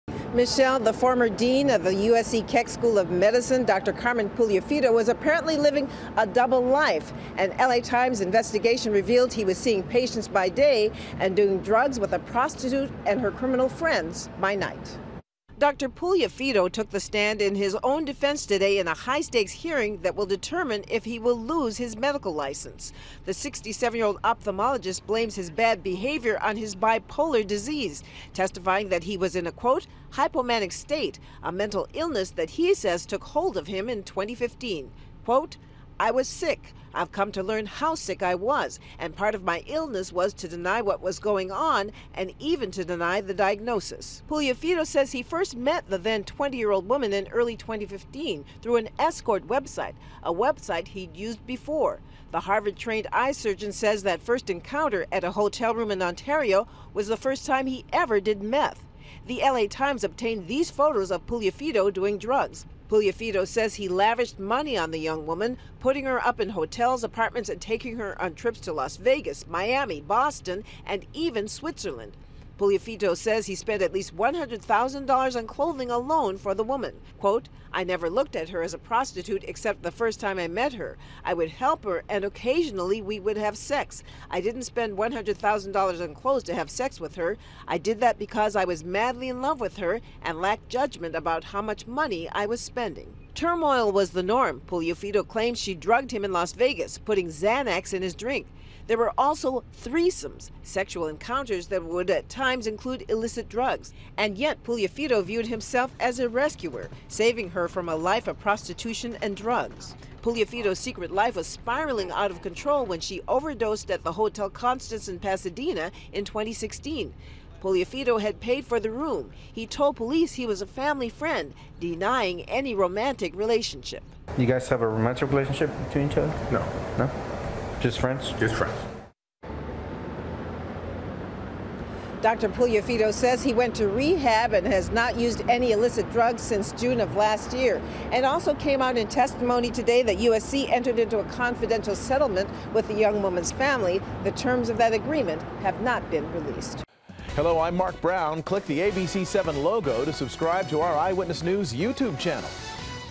Former USC medical school dean blames sickness for bad behavior
Dr. Carmen Puliafito, former dean of USC's medical school, took the stand in his own defense Thursday in a high-stakes hearing that will determine if he will lose his medical license.